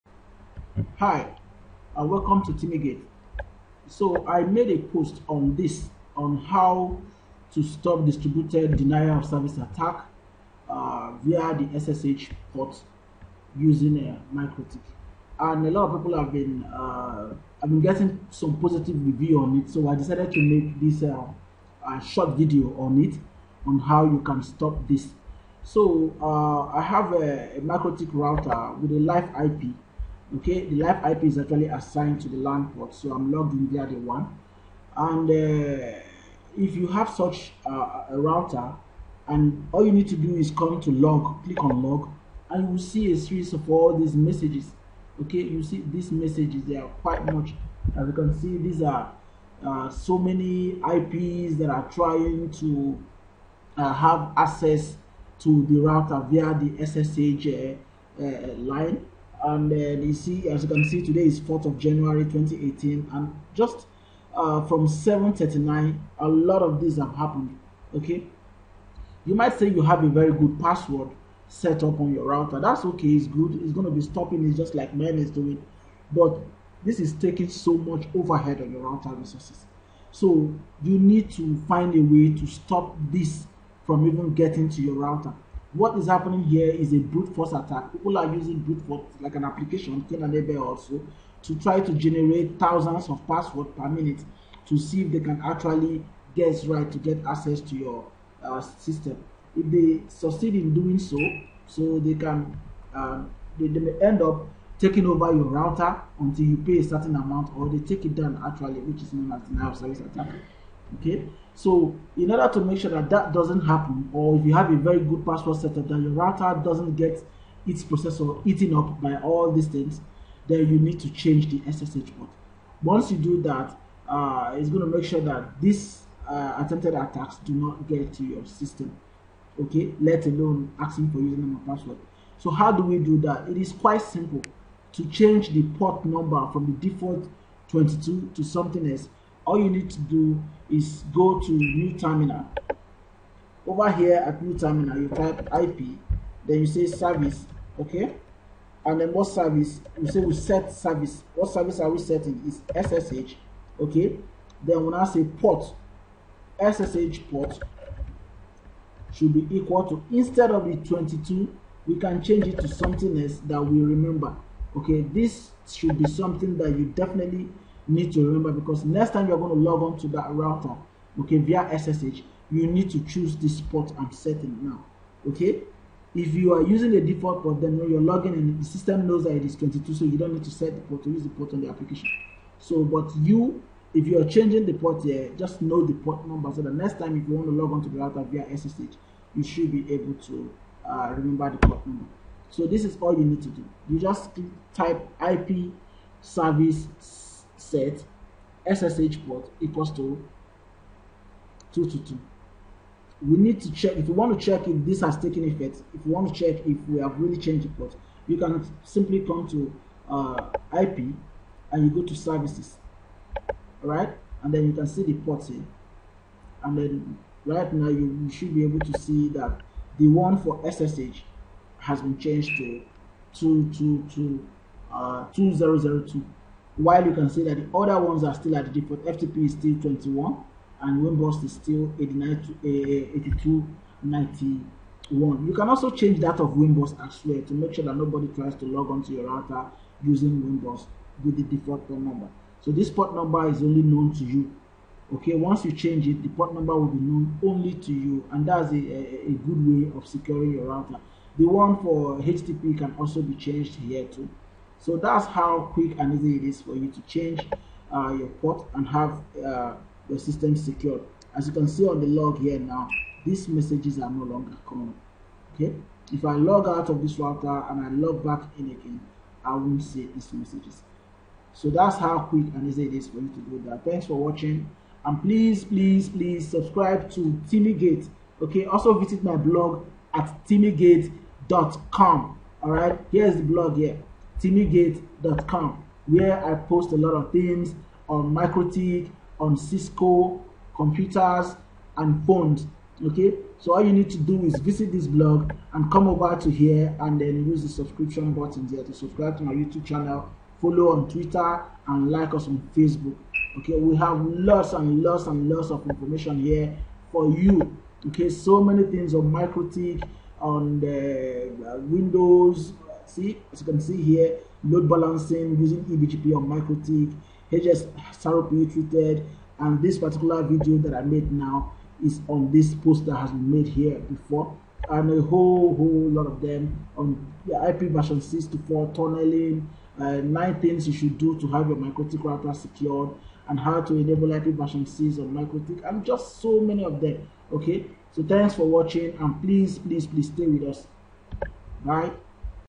How to secure your Mikrotik router from unauthorized access through SSH
If you are using Mikrotik routers on your network with live IPs, you may have come across log messages that notify you of failed login attempts via ssh. Such messages are generated by failed unauthorized ssh attempts on your router by cybercriminals who are constantly trying to access and take down active devices in what is known as distributed denial of service attacks. Visit timigate.com for a blog post on this and others.